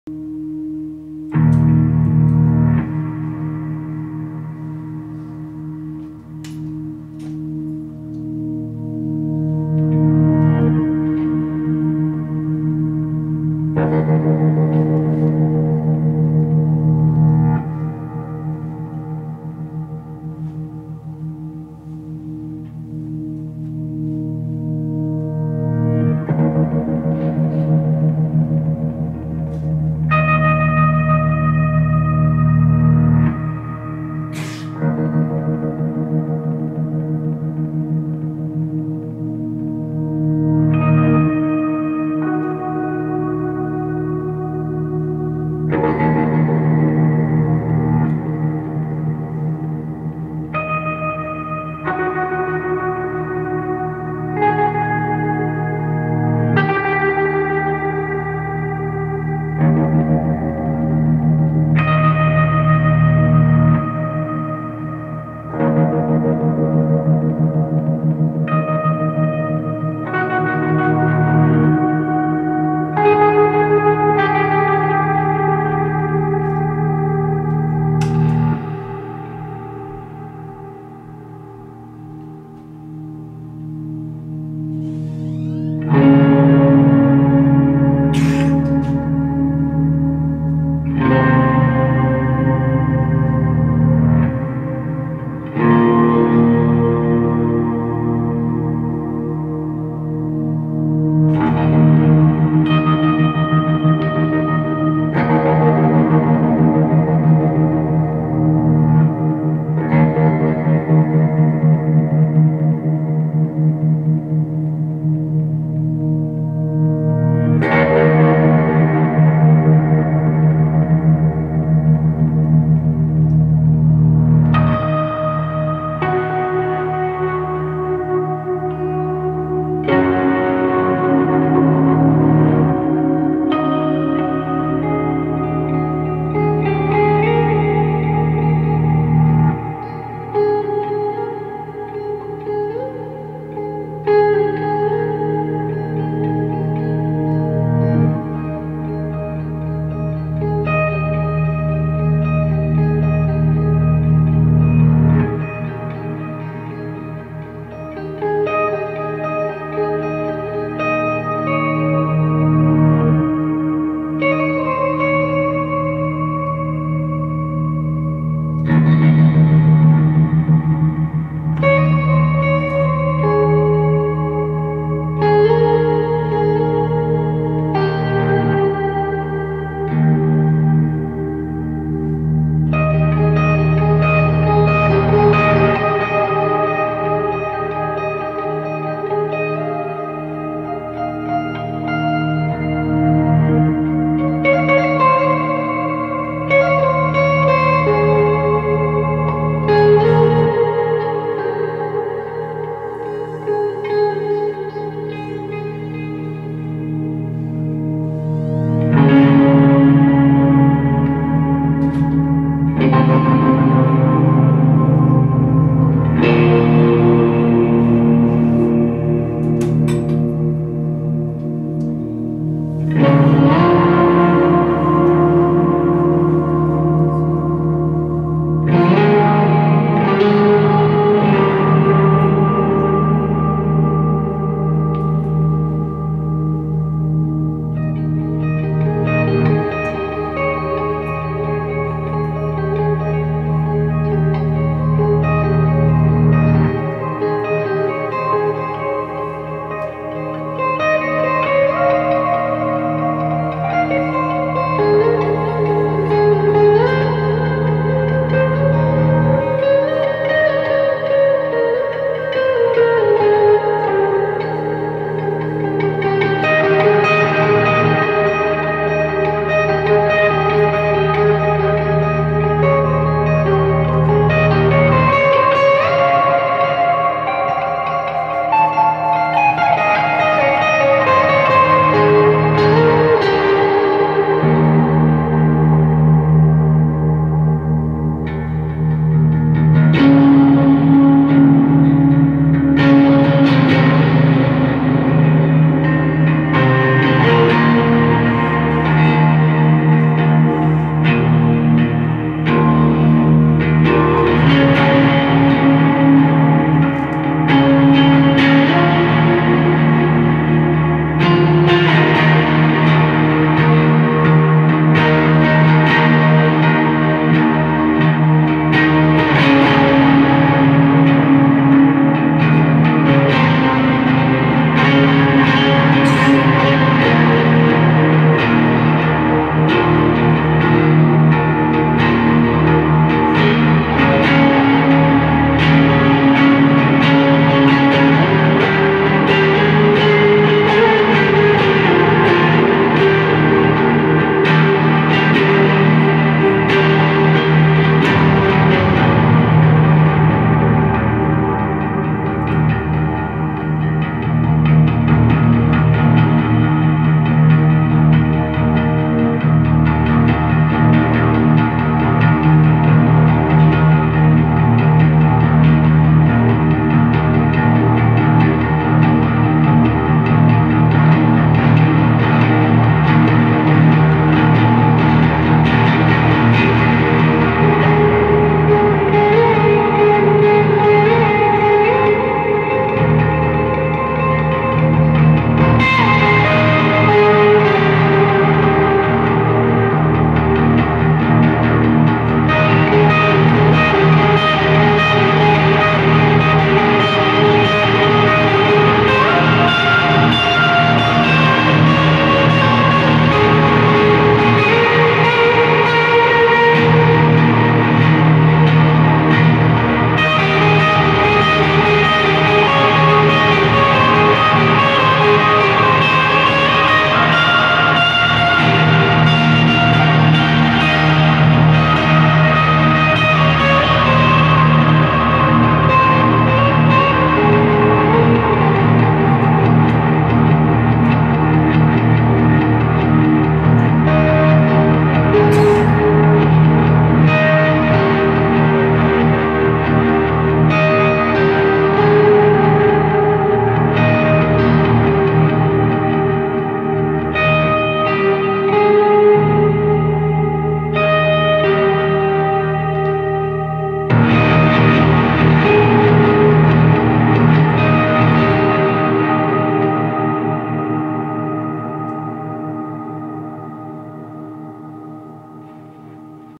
Lachlan R. Dale at Petersham Bowling Club (13/7/2019)
The debut solo performance of Lachlan R. Dale (Hashshashin, Serious Beak, Adrift For Days).